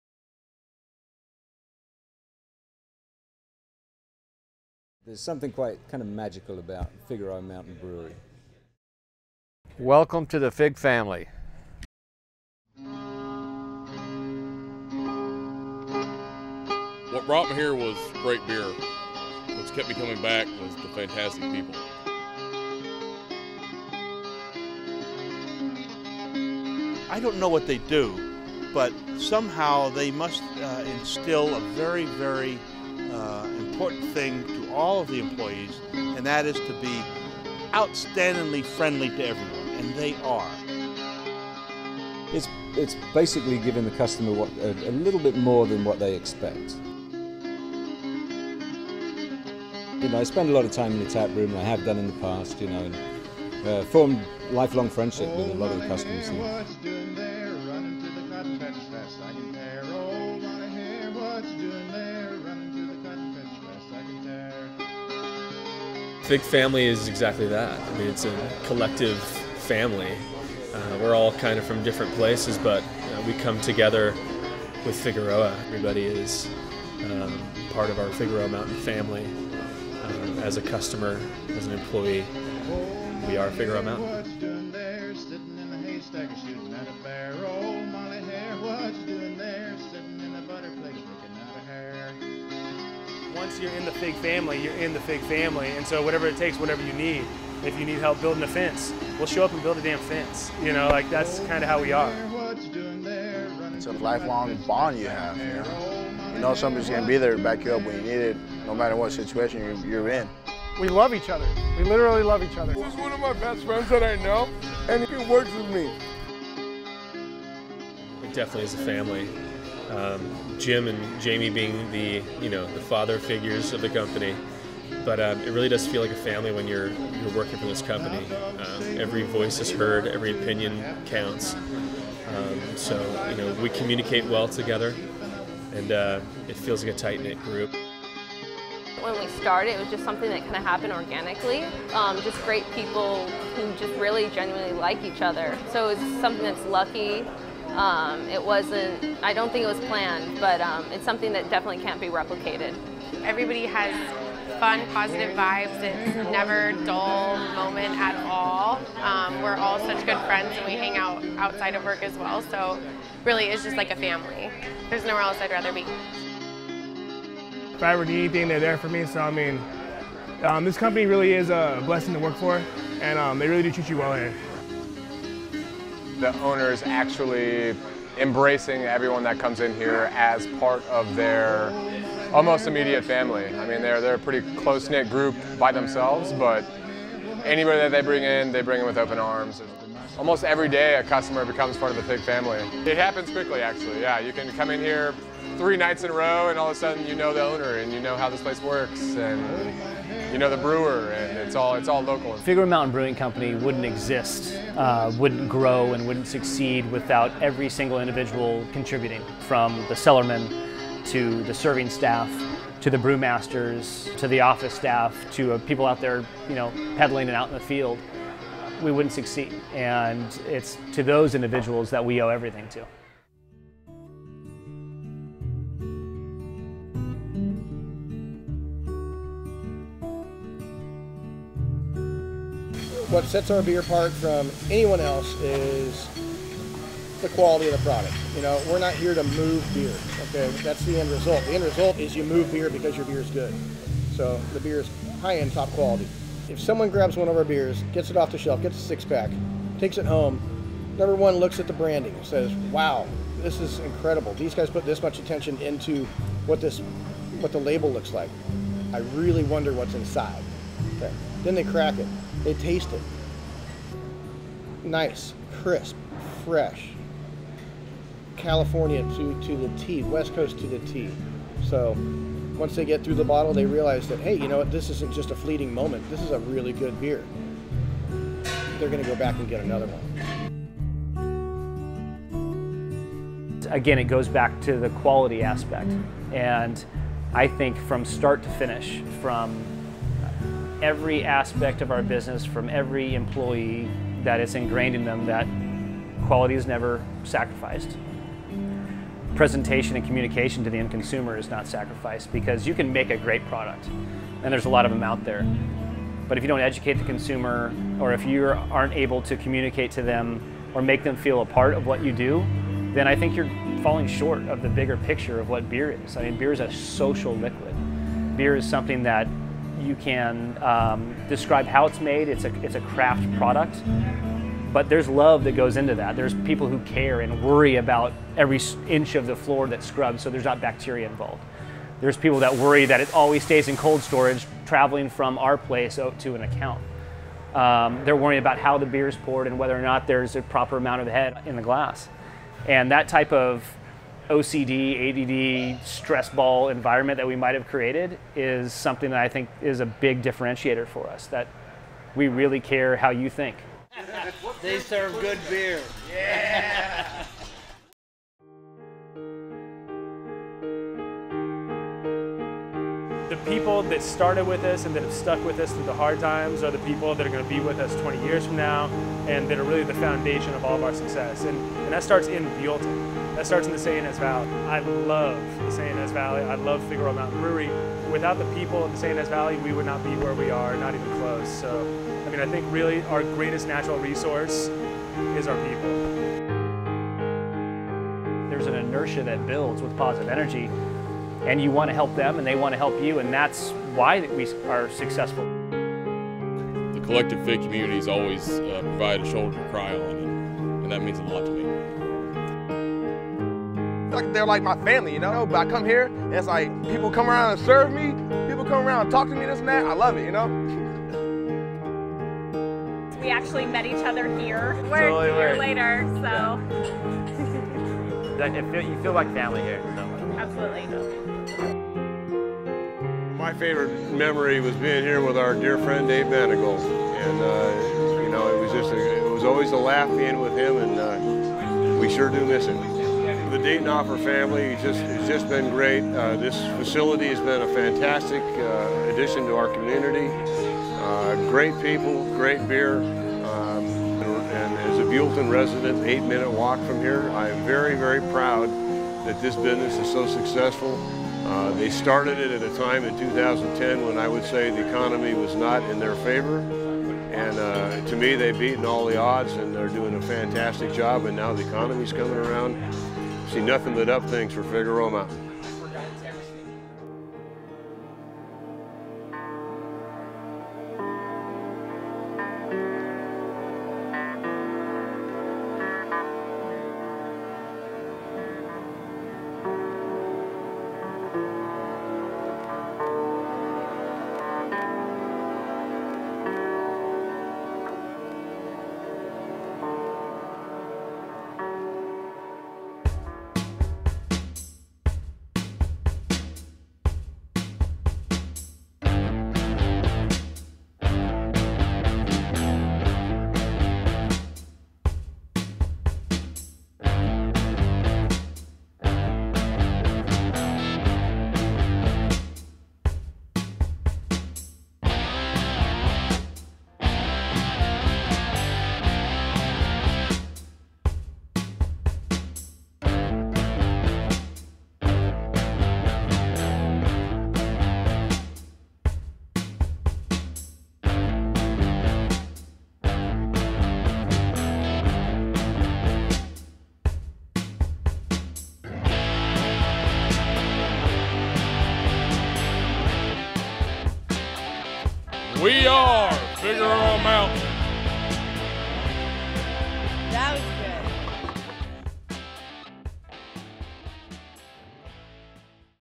WEAREFIGFAMILY
Learn about the FigFam from staff, Mug Club members and customers of the Central Coast brewery, Figueroa Mountain Brewing Co.